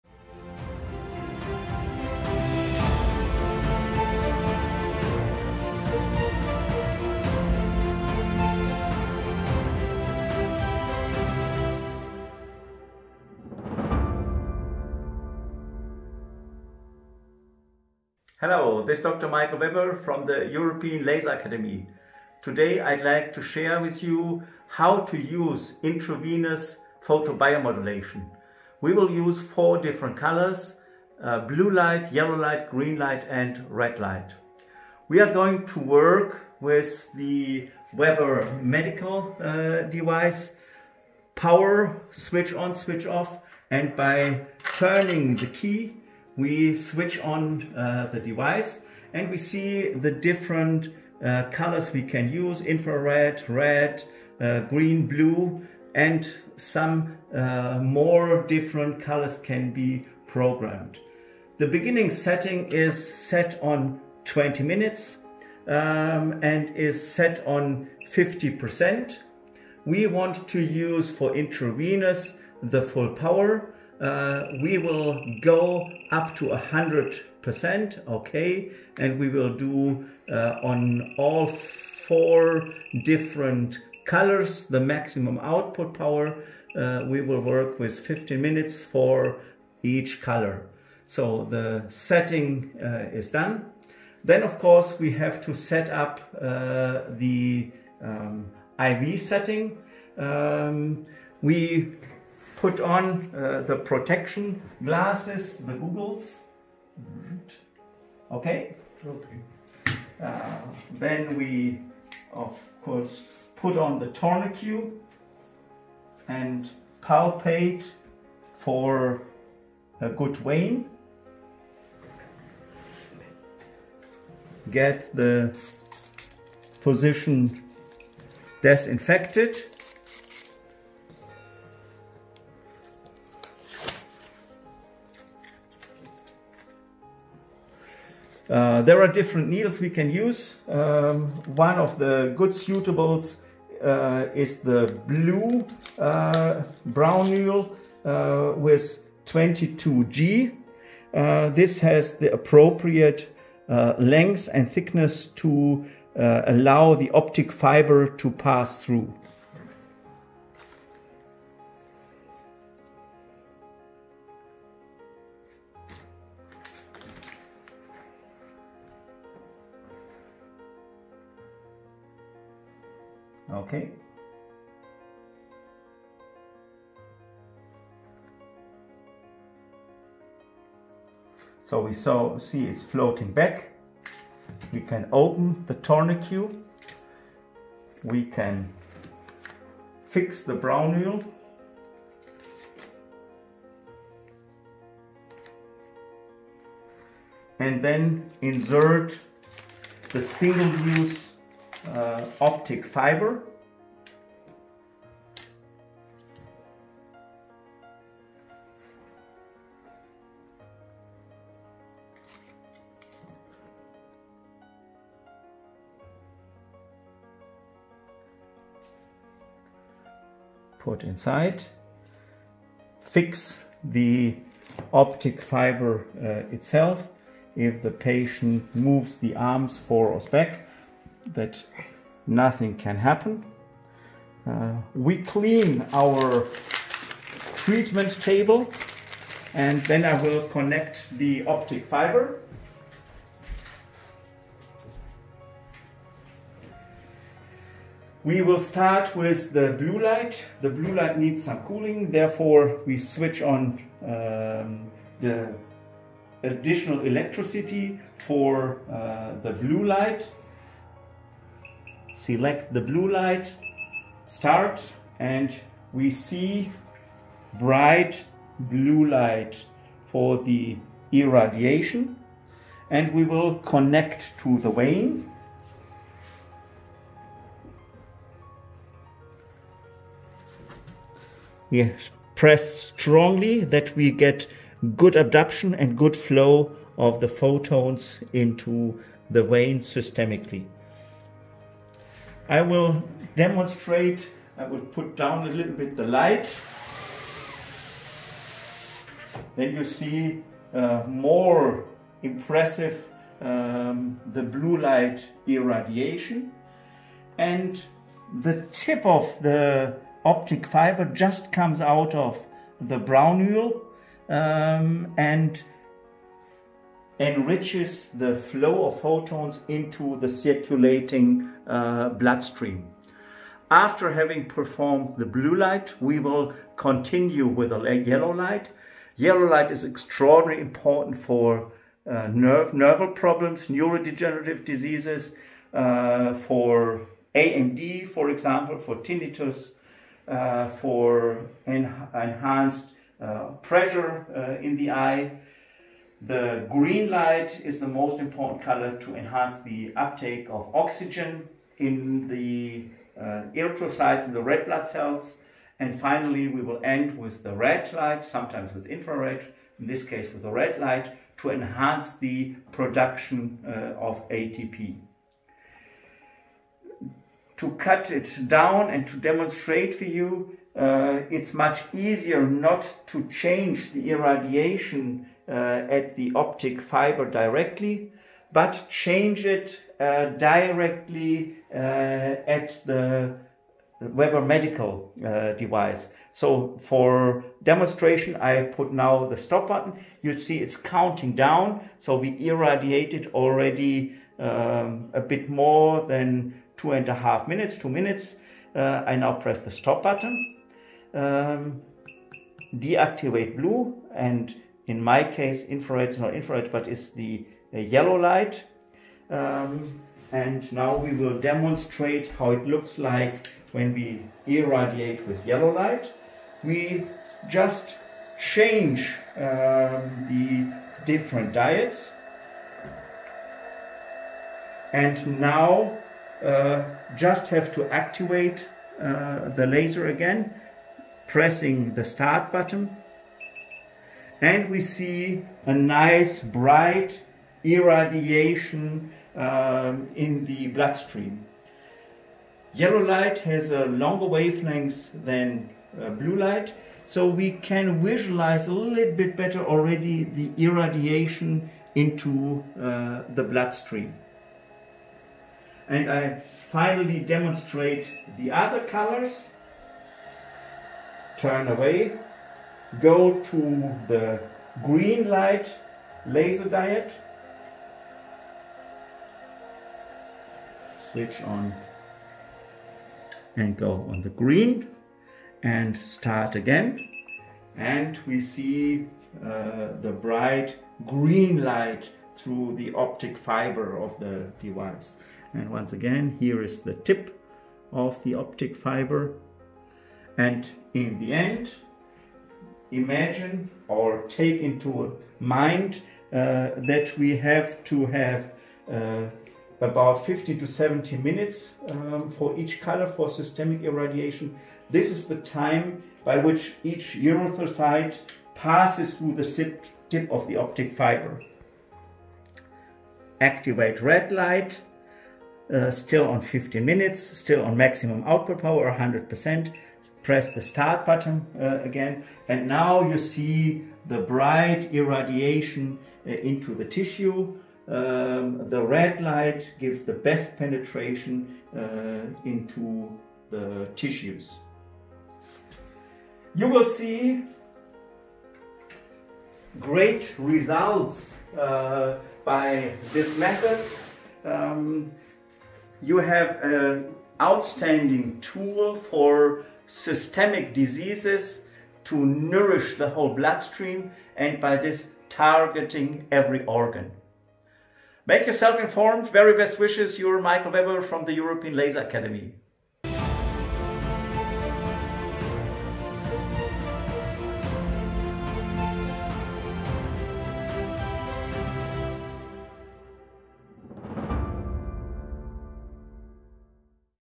Photobiomodulation: PBM by intravenous irradiation of the blood
Intravenous LLLT/ Low Level Laser Therapy with different colors for a huge variety of different diseases